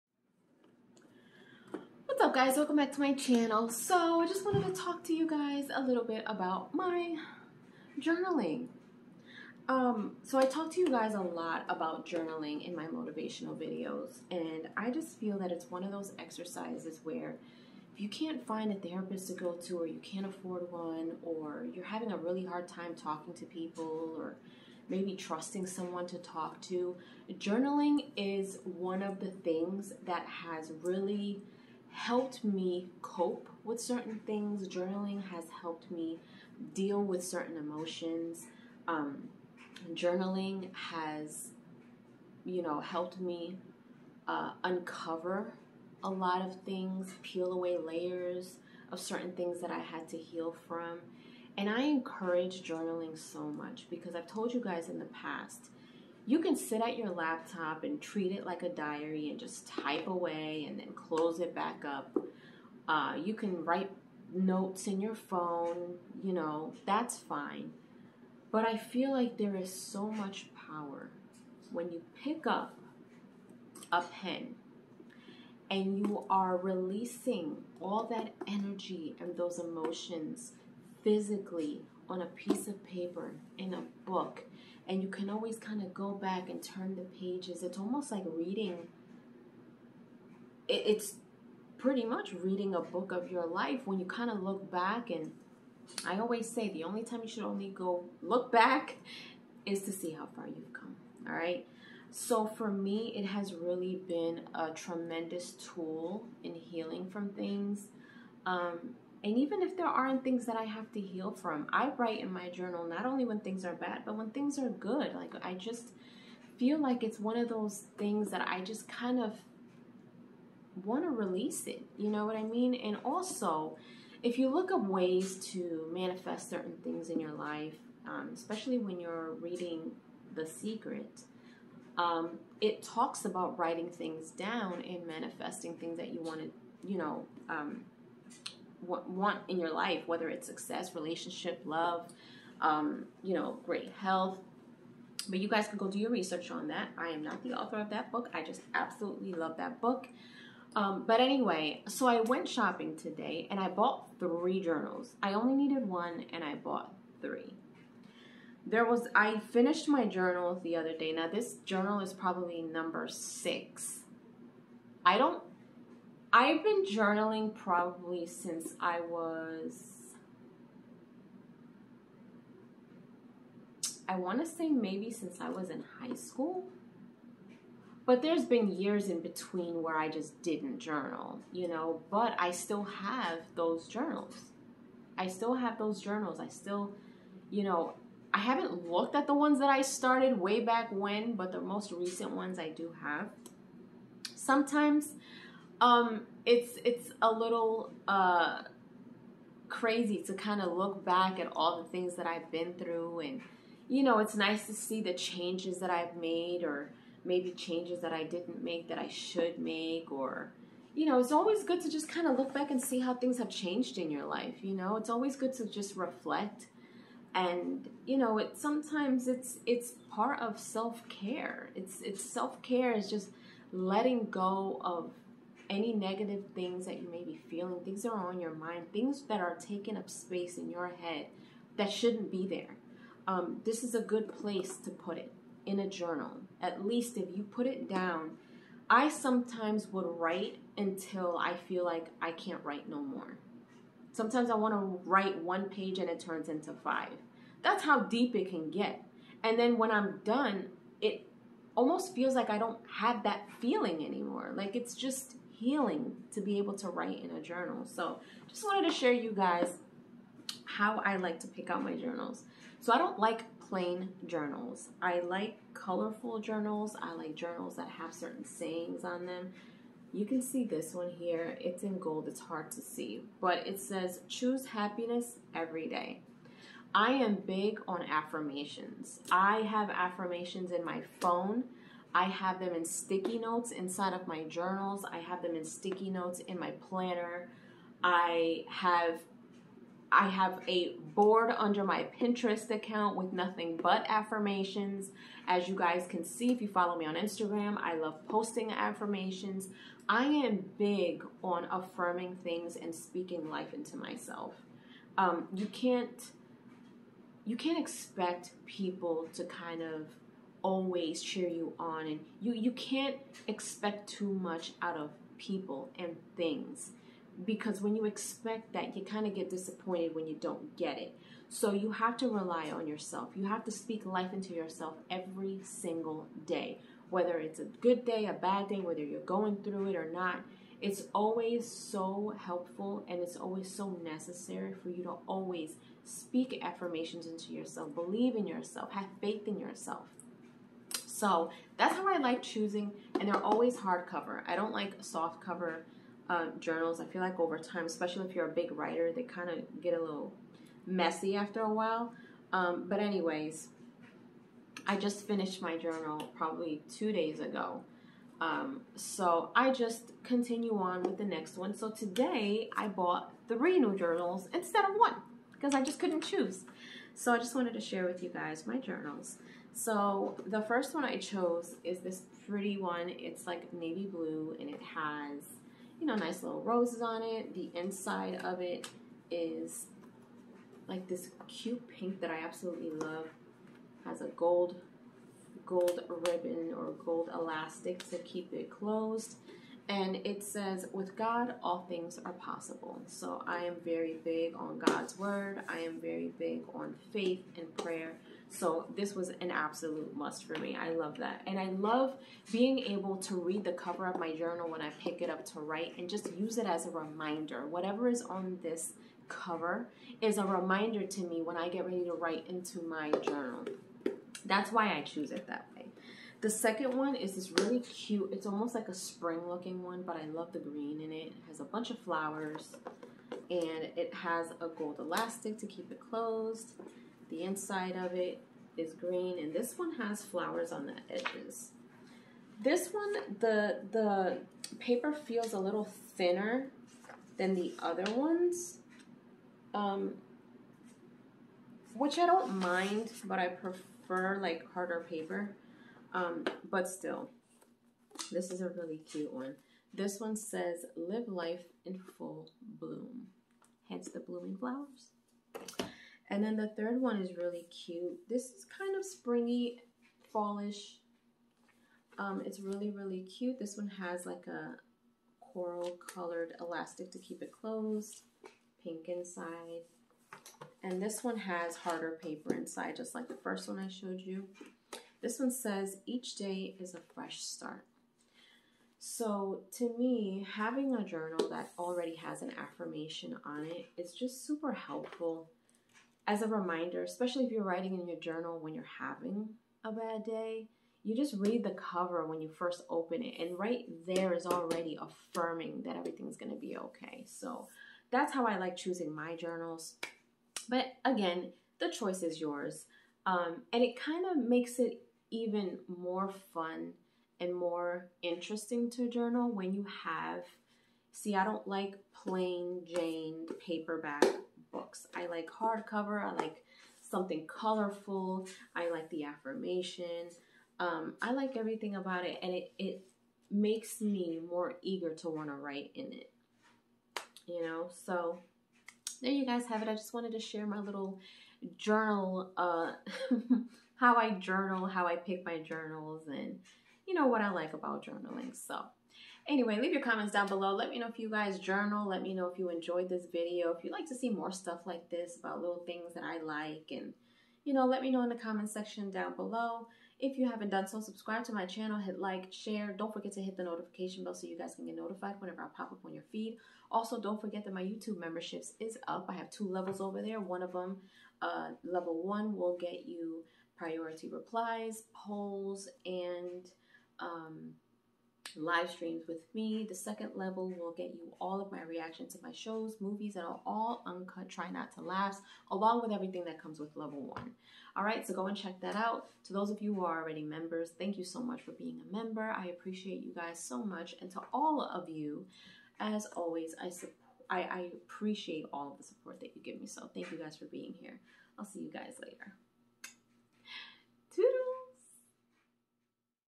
Journal Haul! (One Of My Healing Tools And Why I Choose THESE!)
Check out these cute journals I purchased today and find out how and why I choose them!